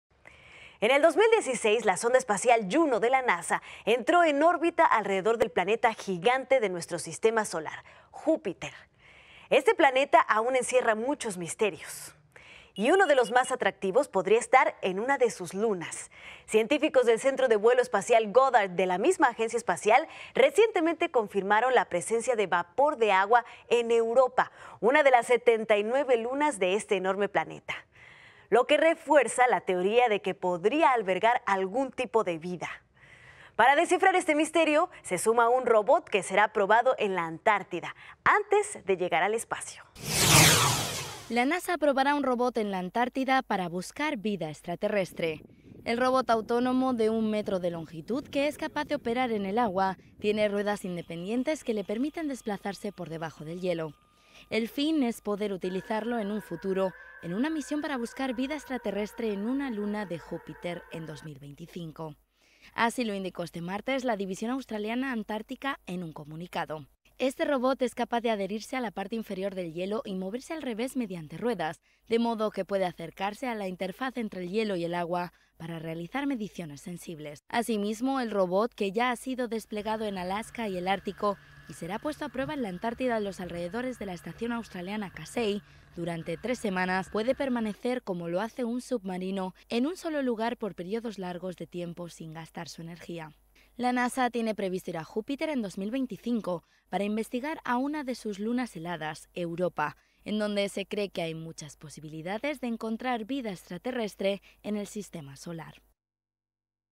La NASA desa­rro­lla un ro­bot para bus­car vida en Eu­ro­pa, la luna de Jú­pi­ter
La Ad­mi­nis­tra­ción Na­cio­nal de la Ae­ro­náu­ti­ca y del Es­pa­cio (NASA) pro­ba­rá un ro­bot para bus­car vida ex­tra­te­rres­tre. El au­tó­ma­ta de un me­tro de lon­gi­tud, ca­paz de ope­rar en el agua, tie­ne rue­das in­de­pen­dien­tes que le per­mi­ten des­pla­zar­se por de­ba­jo del hie­lo.